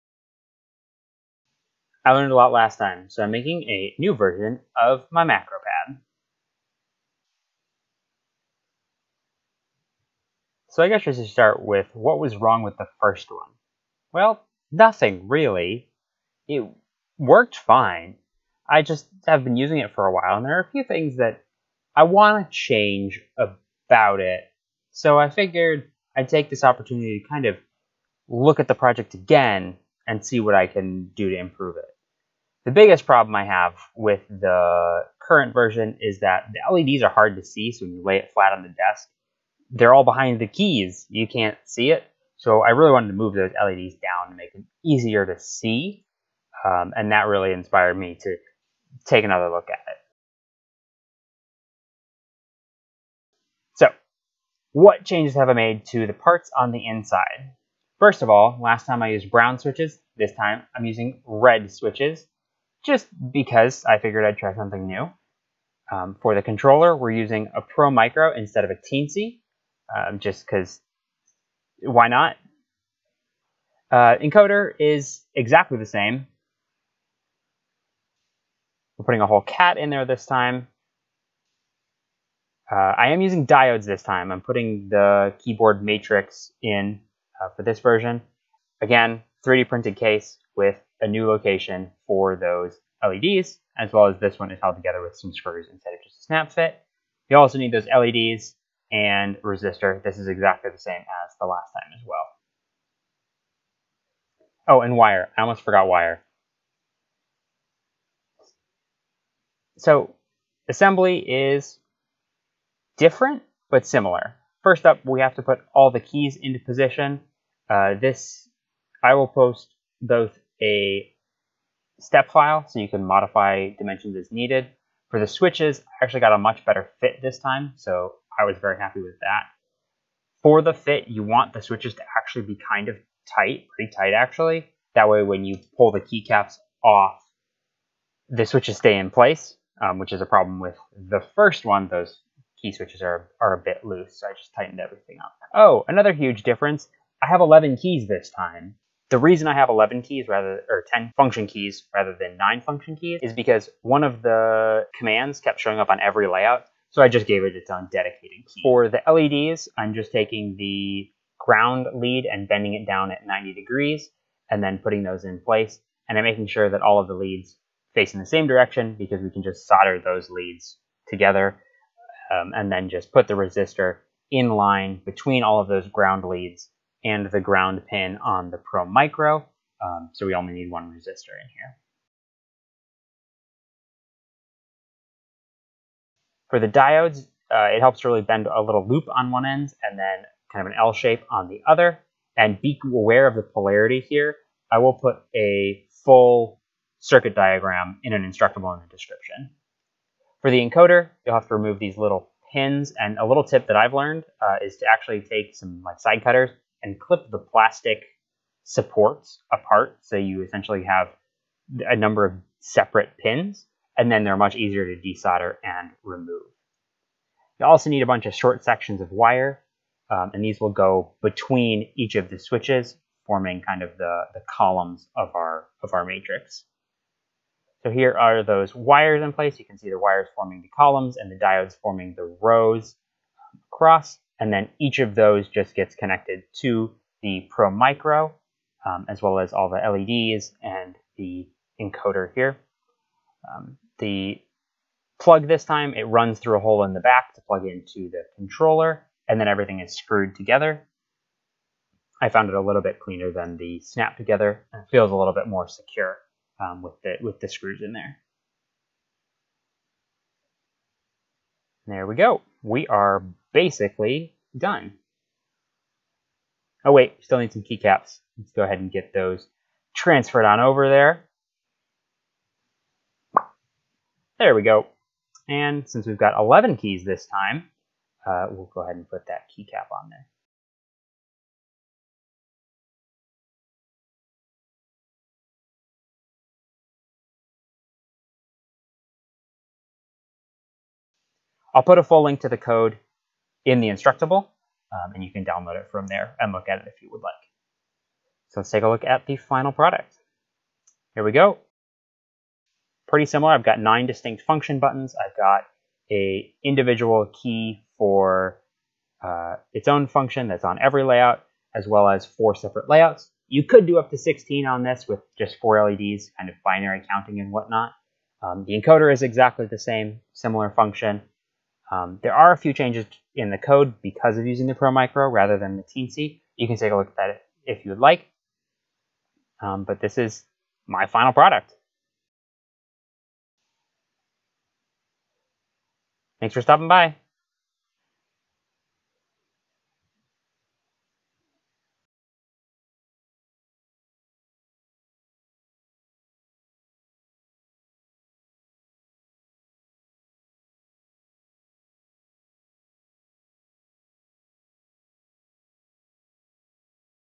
Macropad V2
In this video I go over how I made a new and improved version of my Macropad from the last video. Improvements include: new case design, different switches, different microcontroller, diodes, better code, easier to program (that's up to you I guess but you only need the Arduino IDE for this one).

Parts:
(11) Key Switches
(1) Arduino Pro Micro
(1) Rotary Encoder
(11) IN4148 Diodes
Case & Hardware
(4) LEDs
(1) Resistor: (you probably have one in an arduino kit) otherwise
Wire
USB Cable
Optional:
Rubber Feet